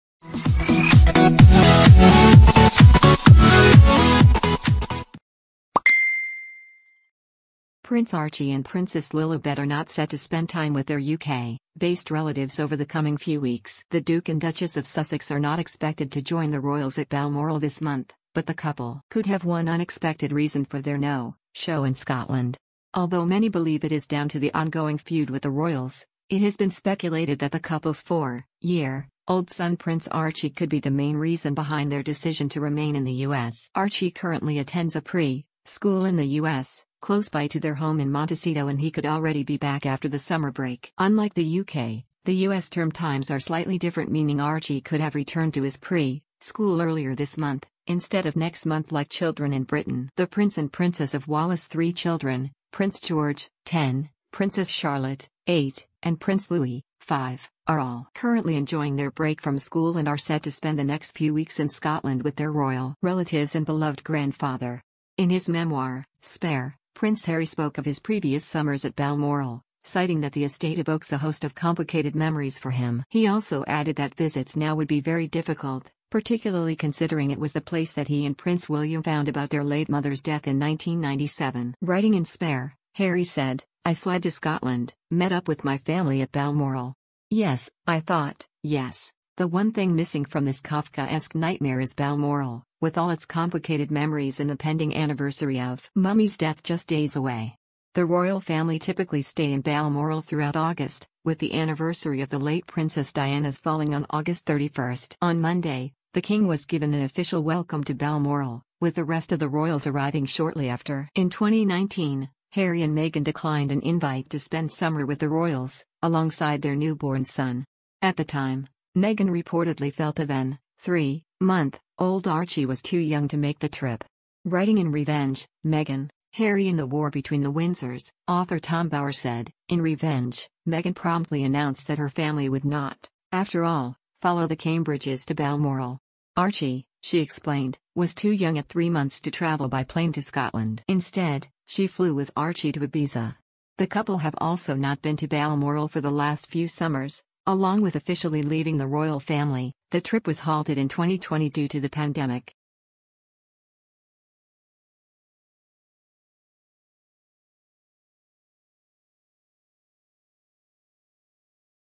Prince Archie could be the real reason behind Harry and Meghan's decision to snub Balmoral
Prince Archie and Princess Lilibet are not set to spend time with their UK-based relatives over the coming few weeks. The Duke and Duchess of Sussex are not expected to join the royals at Balmoral this month, but the couple could have one unexpected reason for their no-sho...